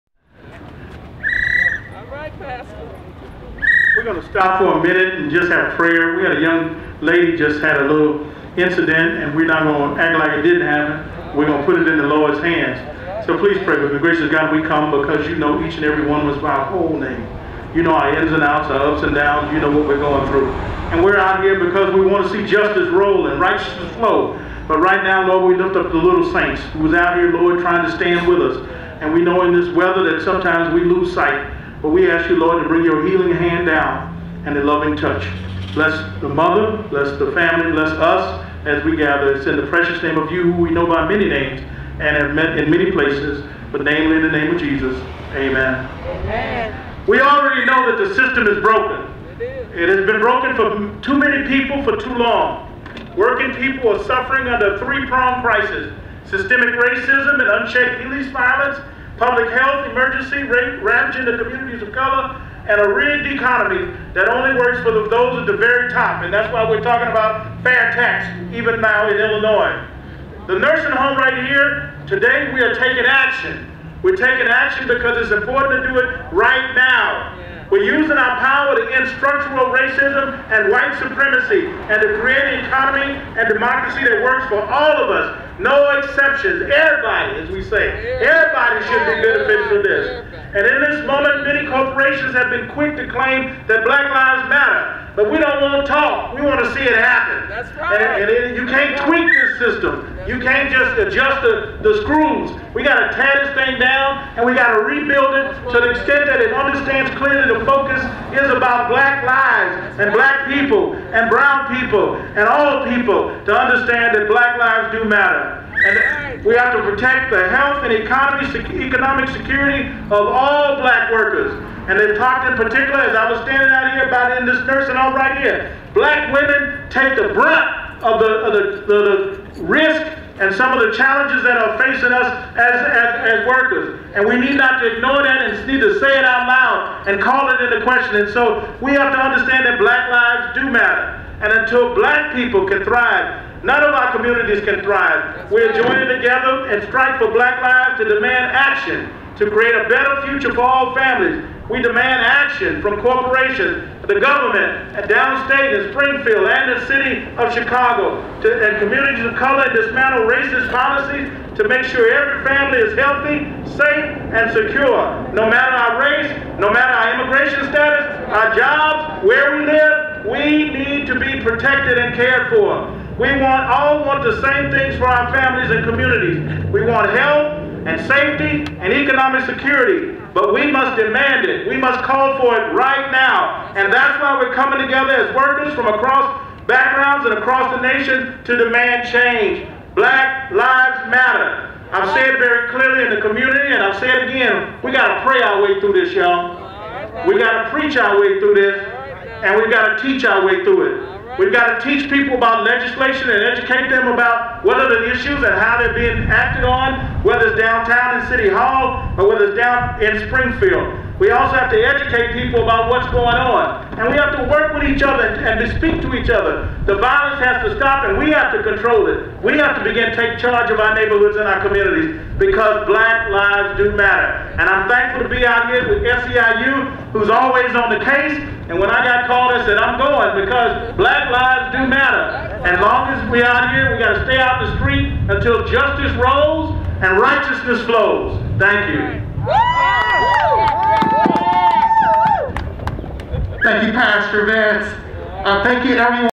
Pastor Booker Vance Speaks at Nursing Home StrikeForBlackLives Event
Pastor Booker Vance: "In this moment, many corporations have been quick to claim that Black Lives Matter, yet have done little to nothing to change policies that would protect the health and economic security of Black workers."